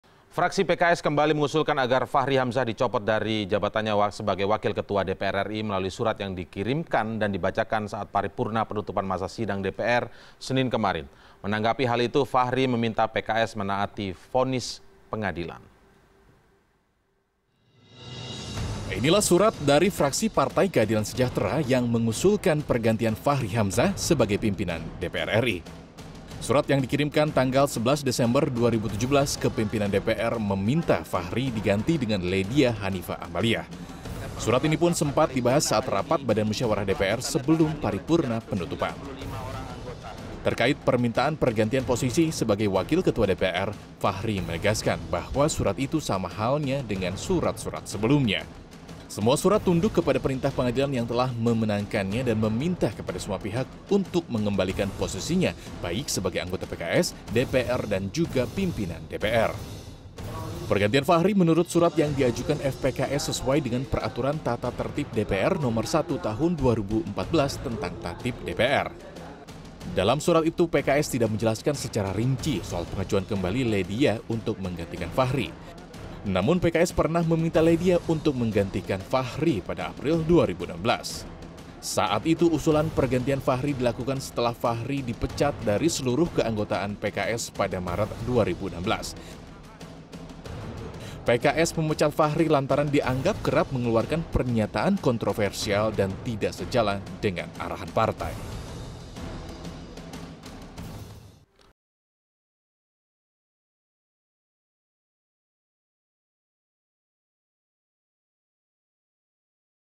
PKS Ajukan Pencopotan Fahri Hamzah Sebagai Pimpinan DPR
Tak hanya Setya Novanto yang mengajukan surat pengunduran diri sebagai Ketua DPR, dan Aziz Syamsuddin ditolak Bamus untuk menggantikannya. Namun juga Fraksi Partai Keadilan Sejahtera, yang kembali mendesak Pimpinan DPR untuk mengevaluasi posisi Fahri Hamzah, sebagai Wakil Ketua DPR. Terbentur masa reses di DPR, surat pergantian Fahri ini harus menunggu sekitar sebulan lamanya hinnga 8 Januari tahun depan.

Like & Follow Facebook:
CNN Indonesia

cnnindonesia